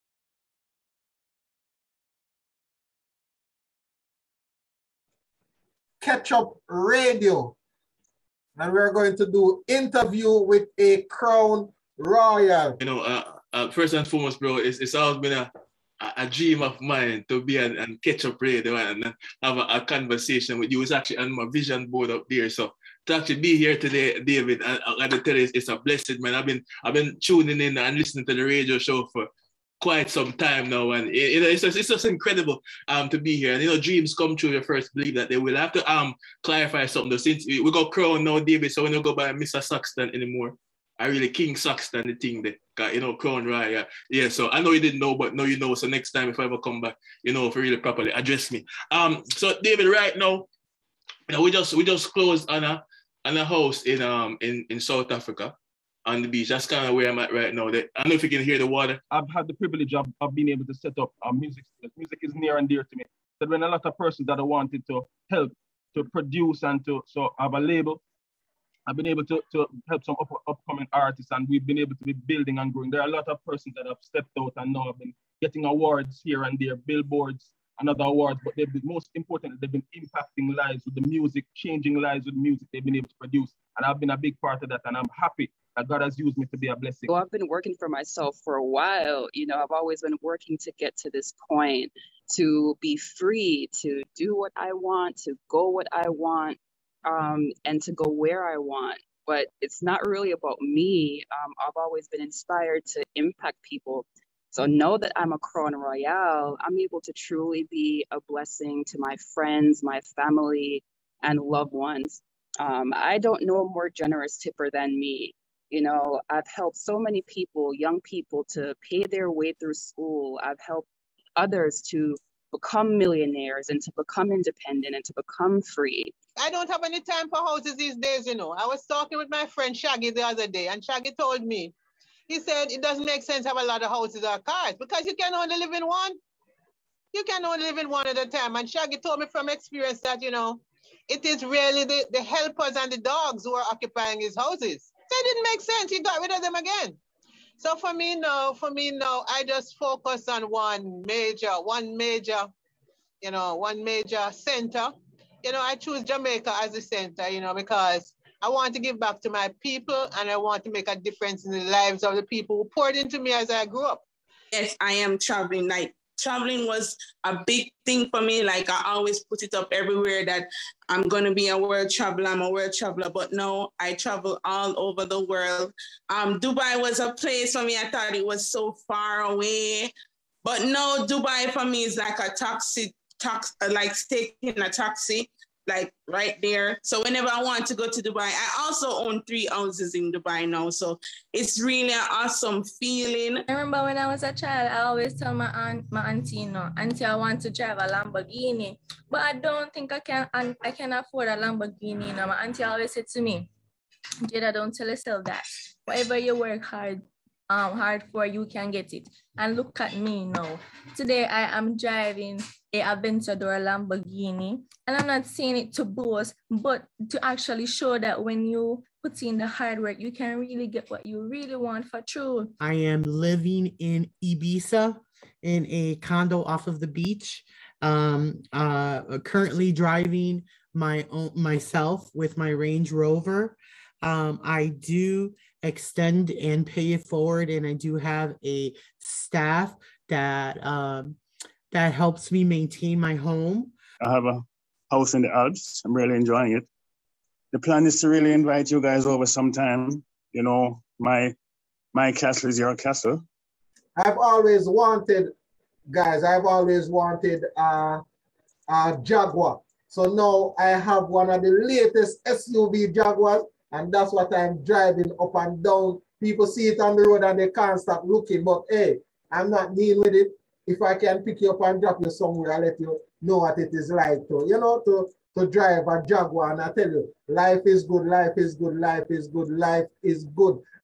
KIDM: INTERVIEW WITH A CROWN ROYALE
T Harv Ecker says your outer world is nothing but a reflection of your inner world. You have to experience it inwardly to experience it outwardly...

You have to live in the future. On KIDM one of our excersizes is Interview with a Crown Royale.

It forces the participants to go to the future in their minds and describe their life.

KETCHUP INNA DI MORNINGS IS 9am EST 8am 🇯🇲 time Mon to Friday